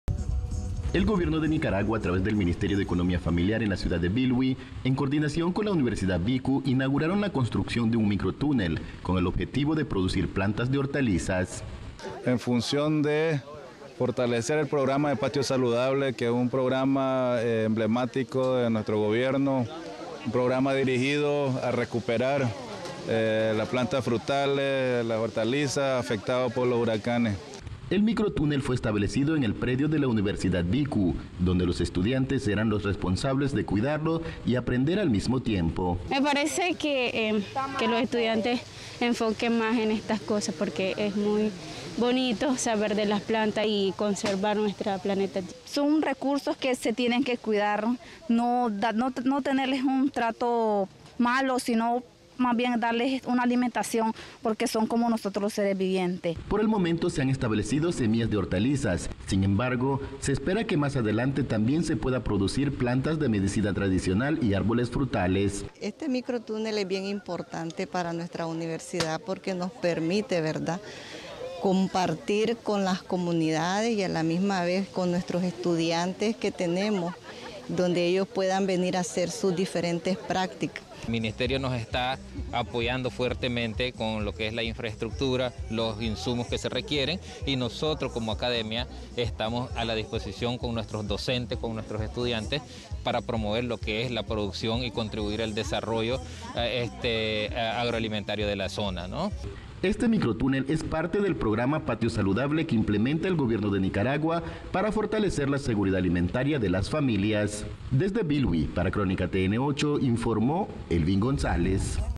Inauguran micro túnel en la Universidad Bicu en Bilwi, Caribe Norte - Nicaragua
El Gobierno de Nicaragua a través del Ministerio de Economía Familiar (MEFCCA) en la ciudad de Bilwi; municipio de Puerto Cabezas, en coordinación con la Universidad BICU inauguraron la construcción de un micro túnel, con el objetivo de producir plantas de hortalizas.

Alli Waters delgado departamental del MEFCCA en su intervención dijo que “este es un programa que nosotros le llamamos patio saludable. Programa que está destinado a fortalecer la seguridad alimentaria de las familias. Queremos rescatar esas costumbres que antes las familias tenían en Bilwi, antes podíamos ver que las familias tenían en sus patios, arboles frutales, plantas medicinales, hortalizas. Por eso este programa tiene ese objetivo de contribuir a la seguridad alimentaria”; expresó Waters en Bilwi.

“Esta zona del país fue afectado por dos huracanes potentes dejando devastación en los cultivos de las familias. Muchos árboles frutales fueron derribados por estos dos fenómenos, es por eso que el Gobierno tiene ese desafío de ir recuperando esos recurso perdidos. Se implementan estos tipos de programas en el casco urbano de Bilwi”; aseveró el funcionario del MEFCCA en Bilwi.

Micro túnel en la Universidad Bicu en Bilwi
“Estos micro túneles aportaran a brindar esos conocimiento de los estudiantes de esta universidad. Nosotros traemos estudiantes de las comunidades de toda la región y es una oportunidad para que estos se apropien de buenos conocimientos que puedan ir a ser reproducidos en sus comunidades. Luego que ingresen de las carreras afines al medio ambiente que impartimos en esta alma mater”; señalo Olga Taylor vice rectora de la Universidad Bicu en Bilwi.

El micro túnel se estableció en el predio de la universidad en donde los estudiantes serán los responsables de cuidarlo y aprender al mismo tiempo.

Por el momento se han establecido semillas de hortalizas sin embargo se espera que mas adelante también se pueda producir plantas de medicina tradicional.